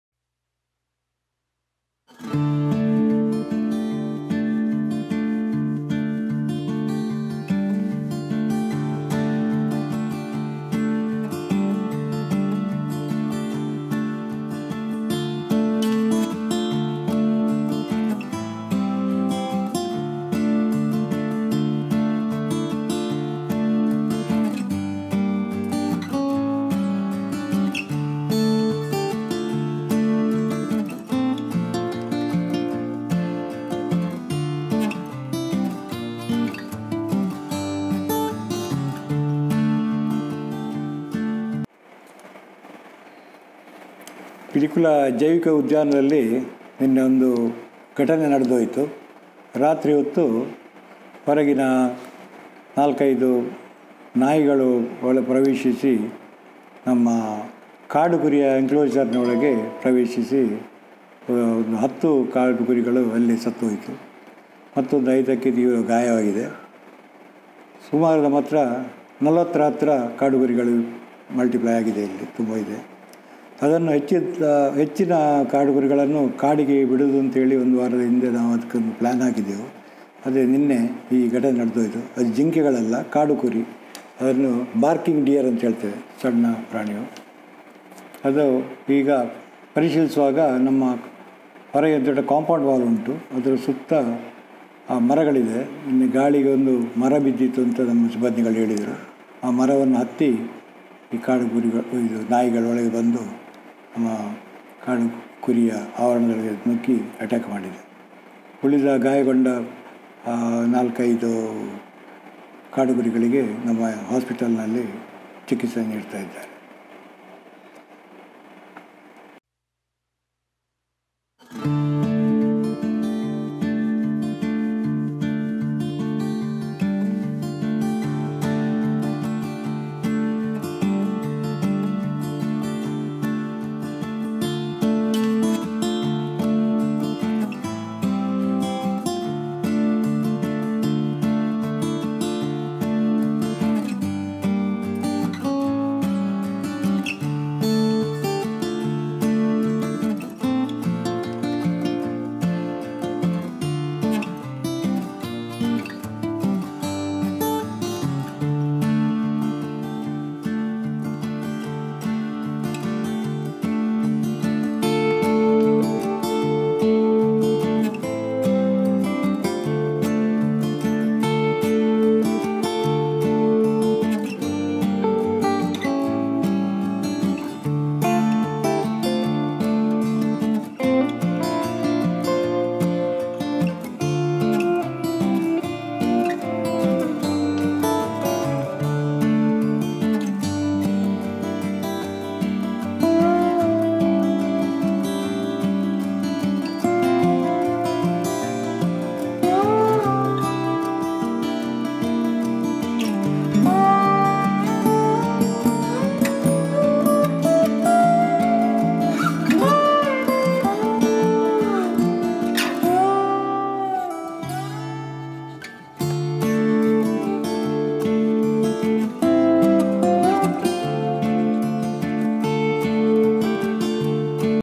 ಪಿಲಿಕುಳದಲ್ಲಿ ಬೀದಿನಾಯಿಗಳ ದಾಳಿಗೆ 10 ಕಾಡುಕುರಿಗಳು ಬಲಿ
► ಬೀದಿ ನಾಯಿಗಳ ದಾಳಿಗೆ ಐದು ಕಾಡು ಕುರಿಗಳ ಸ್ಥಿತಿ ಗಂಭೀರ
► ಜೈವಿನ ಉದ್ಯಾನವನದ ಆಸ್ಪತ್ರೆಯಲ್ಲಿ ಮುಂದುವರಿದ ಚಿಕಿತ್ಸೆ.